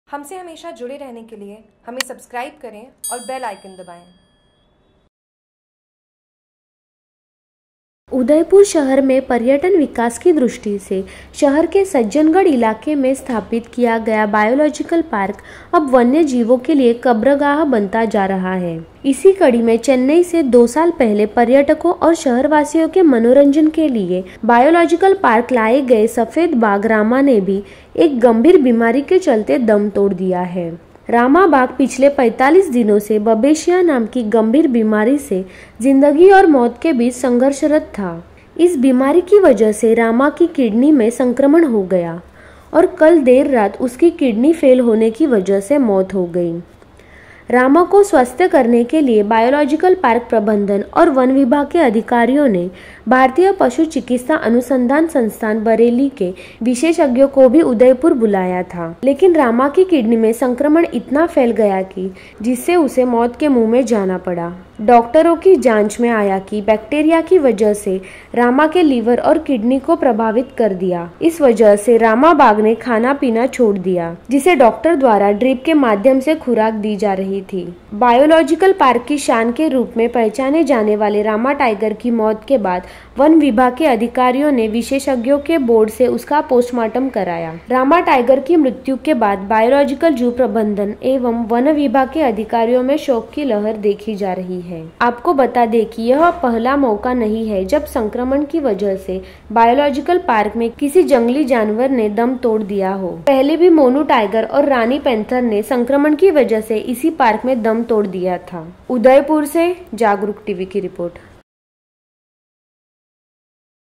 JAGRUK TV : उदयपुर : सफेद बाघ रामा ने तोड़ा दम
उदयपुर शहर में पर्यटन विकास की दृष्टि से शहर के सज्जनगढ़ इलाके में स्थापित किया गया बायलॉजिकल पार्क अब वन्य जीवो के लिए कब्रगाह बनता जा रहा है । इसी कड़ी में चेन्नई से 2 साल पहले पर्यटकों और शहरवासियों के मनोरंजन के लिए बायोलॉजिकल पार्क लाए गए सफेद बाघ रामा ने भी एक गम्भीर बीमारी के चलते दम तोड़ दिया है । रामा बाघ पिछले 45 दिनों से बबेशिया नाम की  गंभीर बीमारी से जिंदगी और मौत के बीच संघर्षरत था । इस बीमारी की वजह से रामा की किडनी में संक्रमण हो गया और कल देर रात उसकी किडनी फेल होने की वजह से मौत हो गयी । रामा को स्वस्थ करने के लिए बायोलॉजिकल पार्क प्रबंधन और वन विभाग के अधिकारियों ने भारतीय पशु चिकित्सा अनुसंधान संस्थान बरेली के विशेषज्ञों को भी उदयपुर बुलाया था लेकिन रामा की किडनी में संक्रमण इतना फैल गया की जिससे उसे मौत के मुँह में जाना पड़ा । रामा भाग के अस्वस्थ होने के बाद से ही जयपुर जोधपुर और उदयपुर के बायो पार्क के डॉक्टर द्वारा उसकी जांच कर इलाज शुरू किया गया था । डॉक्टरों की जांच में आया कि बैक्टीरिया की वजह से रामा के लीवर और किडनी को प्रभावित कर दिया । इस वजह से रामा बाघ ने खाना पीना छोड़ दिया जिसे डॉक्टर द्वारा ड्रिप के माध्यम से खुराक दी जा रही थी । रामा उदयपुर में लाया जाने वाला पहला सफेद बाघ था जिसे अक्टूबर 2016 में अरीगनर अन्ना जूलॉजिकल पार्क चेन्नई से उदयपुर के बायोलॉजिकल पार्क में लाया गया था ।